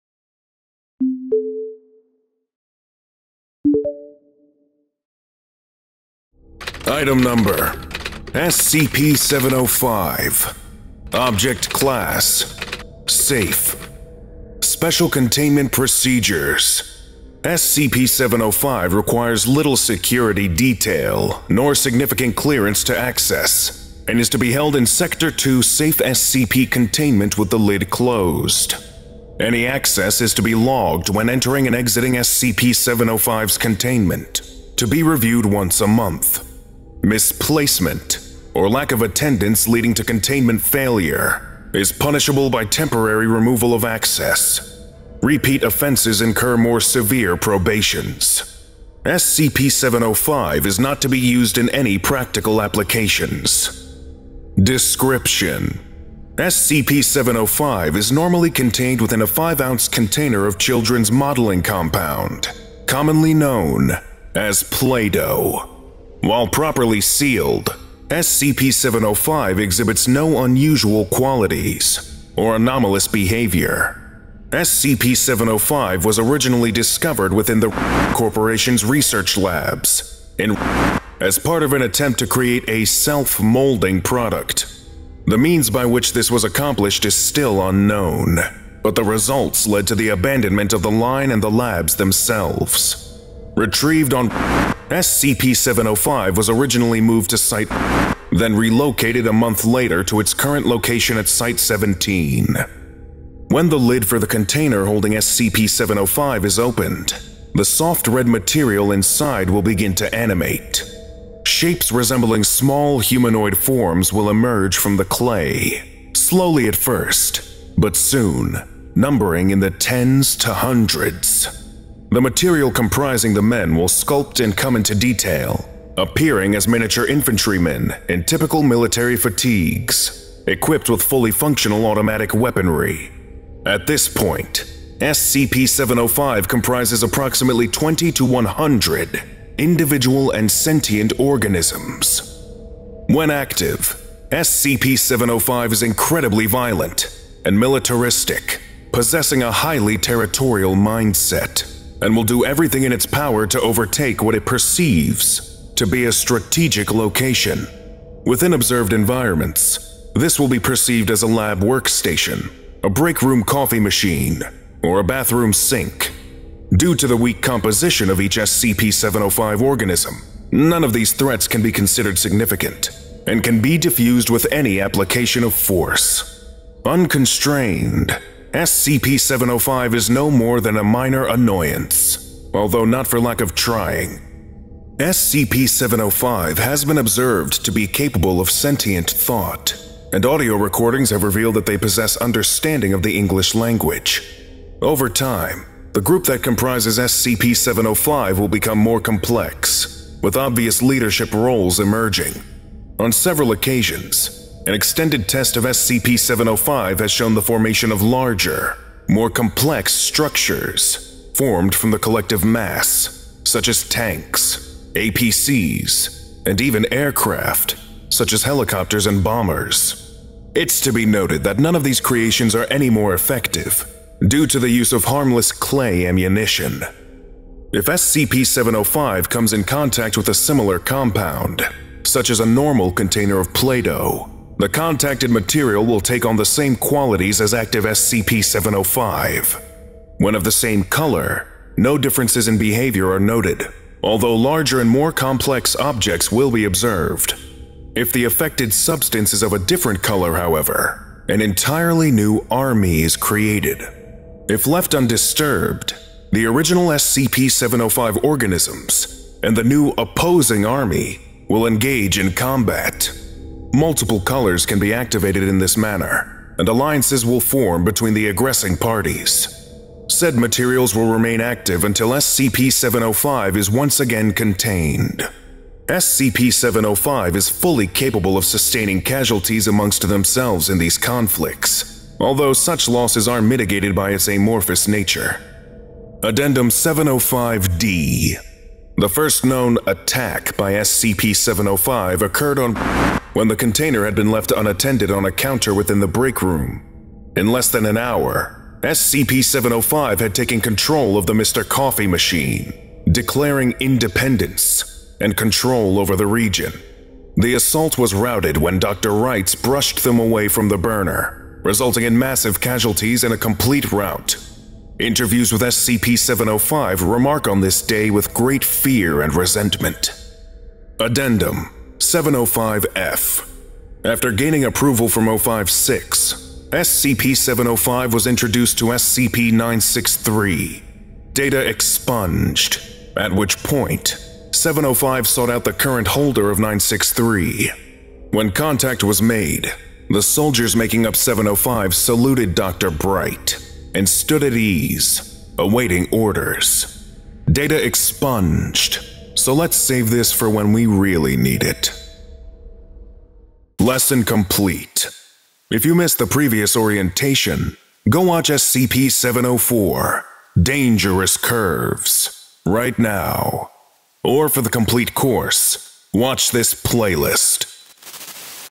SCP-705 | Militaristic Play-Doh (SCP Orientation)
Today we will be studying Item number SCP-705: Militaristic Play-Doh, Object class: Safe.

SCP-705 is normally contained within a 5 oz container of children’s modeling compound, commonly known as “Play-Doh”.

When the lid for the container holding SCP-705 is opened, the soft red material inside will begin to animate. Shapes resembling small, humanoid forms will emerge from the clay, slowly at first but soon numbering in the tens to hundreds. The material comprising the men will sculpt and come into detail, appearing as miniature infantrymen in typical military fatigues, equipped with fully functional automatic weaponry.

When active, SCP-705 is incredibly violent and militaristic, possessing a highly territorial mindset, and will do everything in its power to overtake what it perceives to be a “strategic” location.

and released under Creative Commons Sharealike 3.0. Contributor: Dr Kondraki

Author : SymmetryFilms